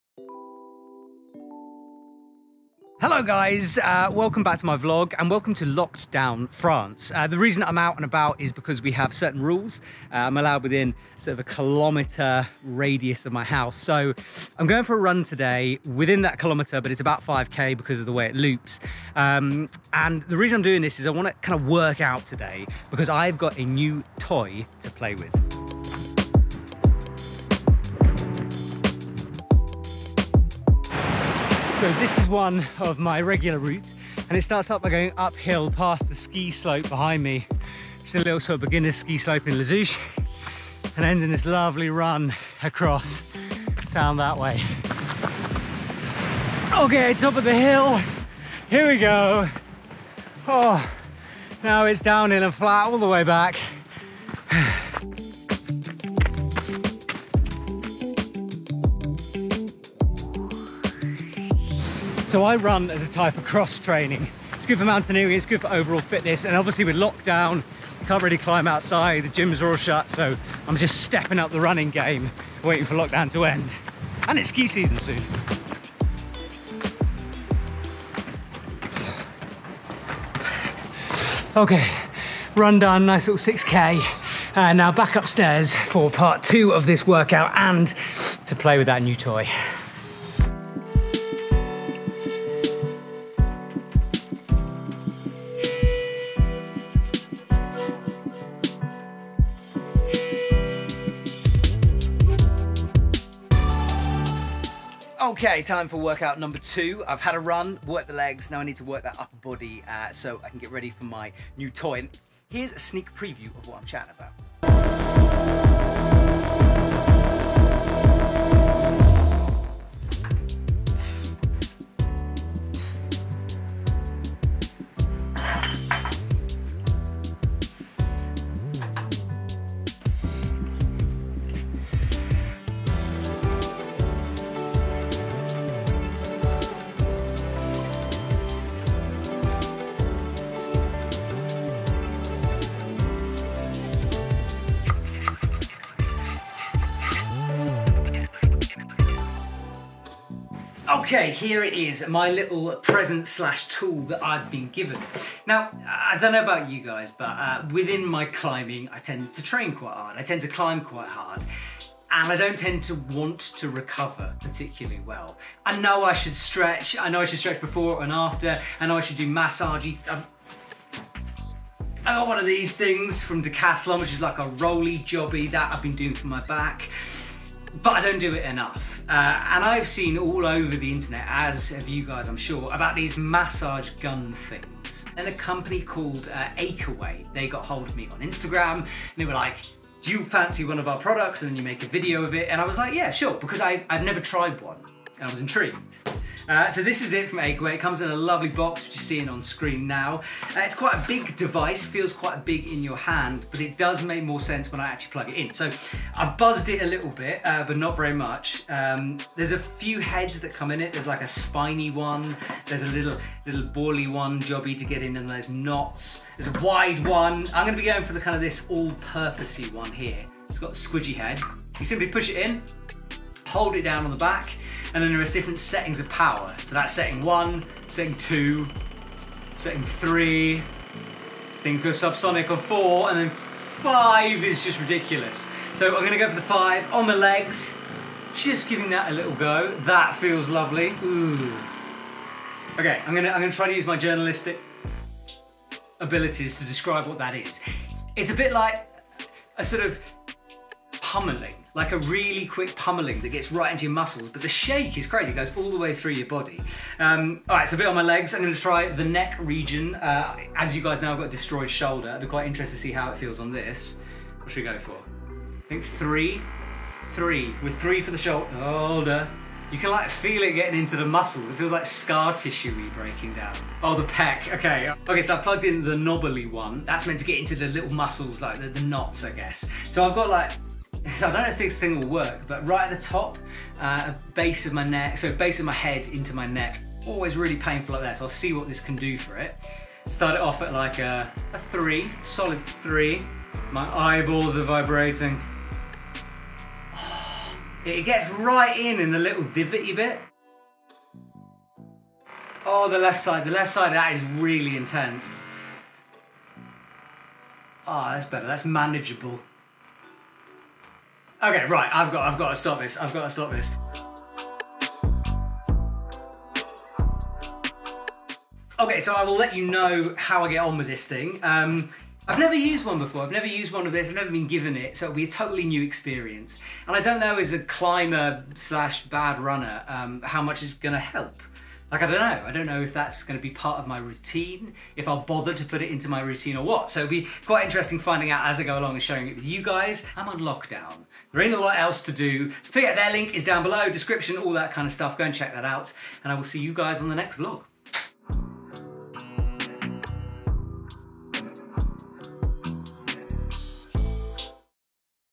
Does A Massage Gun Work For Climbers?
Lockdown 2.0 is underway in France so it's back to running and fingerboarding. I was sent a massage gun by Achedaway so let's see how it works fo climbers!